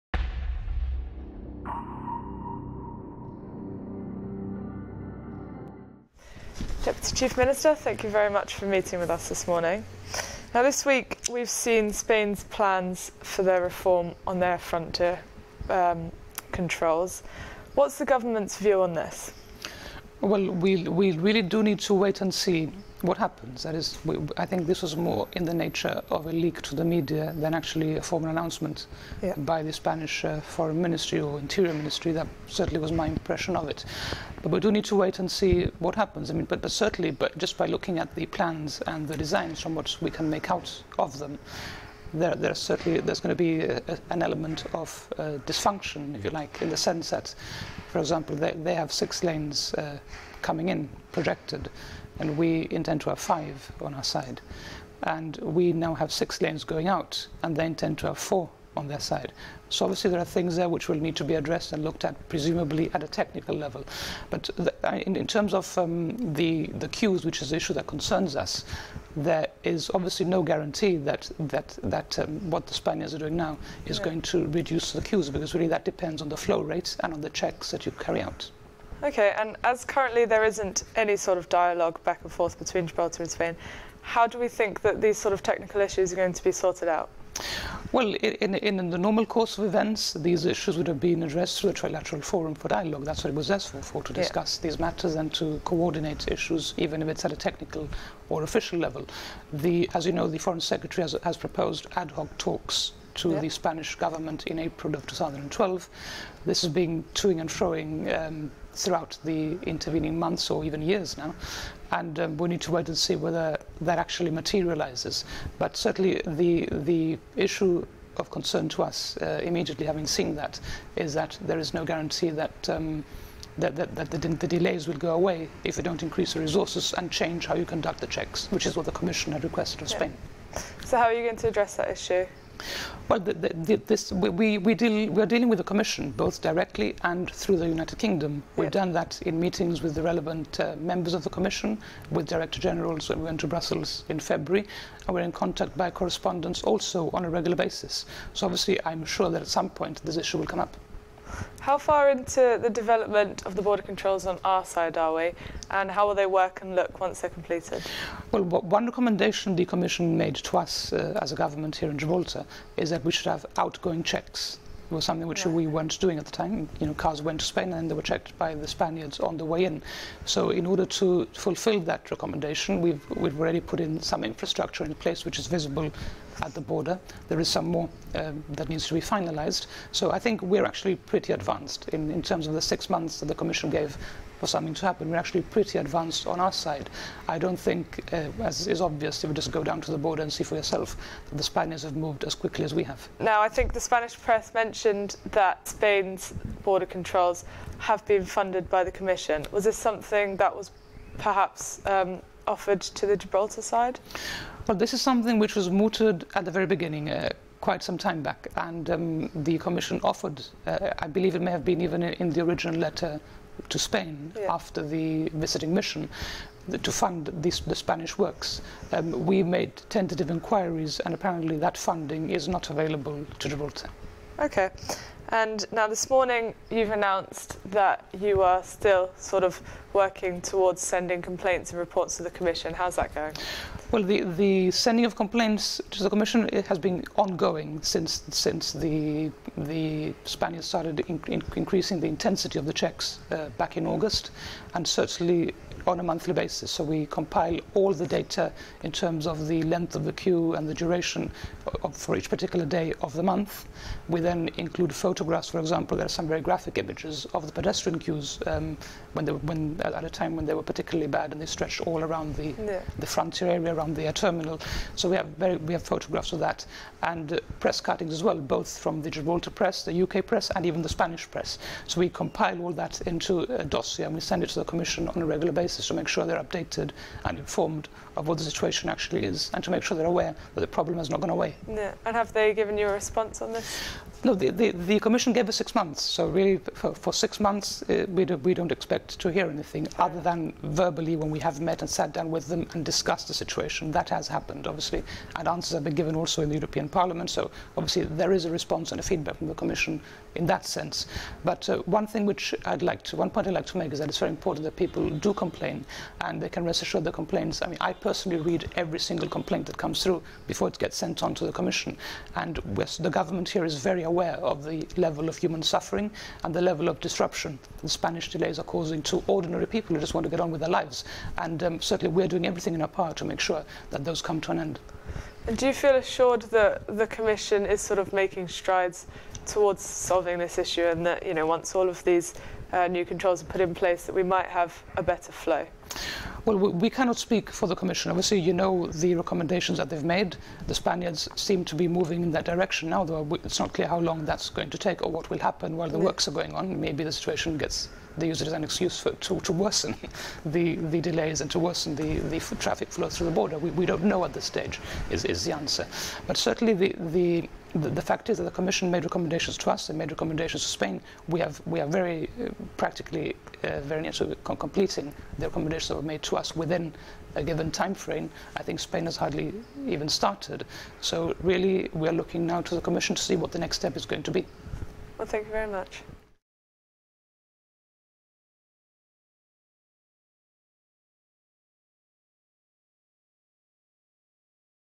YGTV Gibraltar News Video: Gibraltar Frontier Controls Near Completion DCM tells YGTV
Following a leak to the Spanish media this week outlining the Spanish Government's frontier control plans in response to the European Commission's recommendations, YGTV met with Deputy Chief Minister Dr Joseph Garcia for the Government's reactions to Spain's plans for a six lane entrance and four lane exit from Gibraltar. We also spoke of the Commission's offer to fund Spain's implementations and how developed Gibraltar's works at the frontier are...